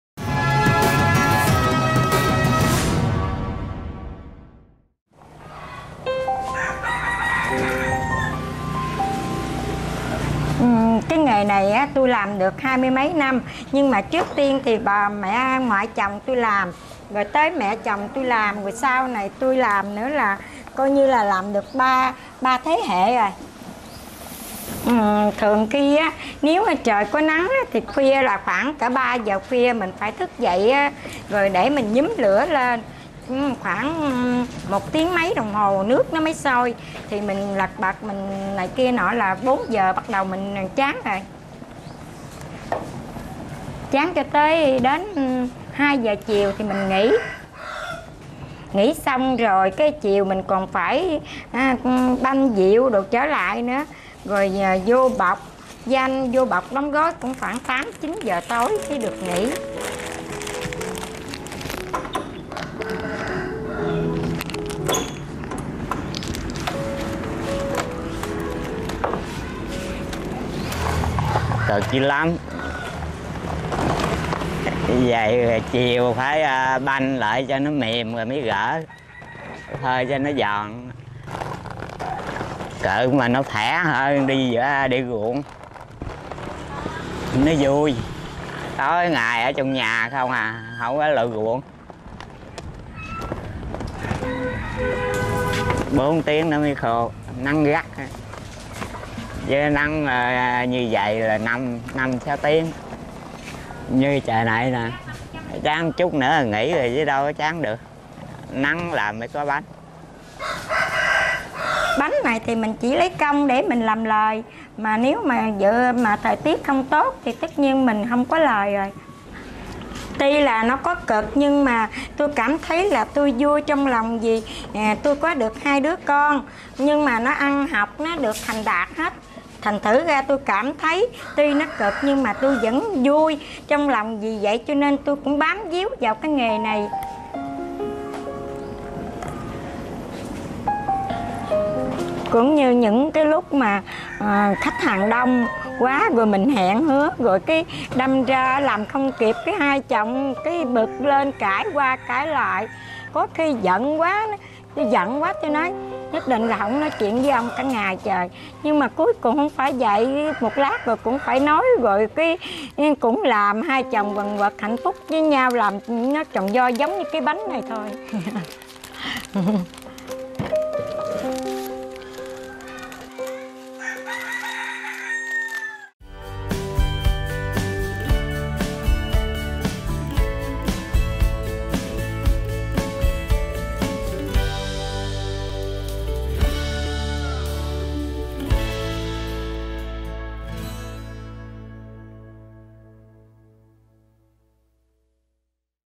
Giữ nghề làm bánh tráng truyền thống ở Cù lao Mây | VTV4
Làng nghề bánh tráng Cù lao Mây hình thành cách đây hơn 100 năm và được xem là một trong những đặc sản làng nghề của tỉnh Vĩnh Long. Bánh tráng Cù lao Mây được khách hàng ưa chuộng bởi cách làm thủ công, với vị ngon truyền thống, mang đậm hương vị quê hương.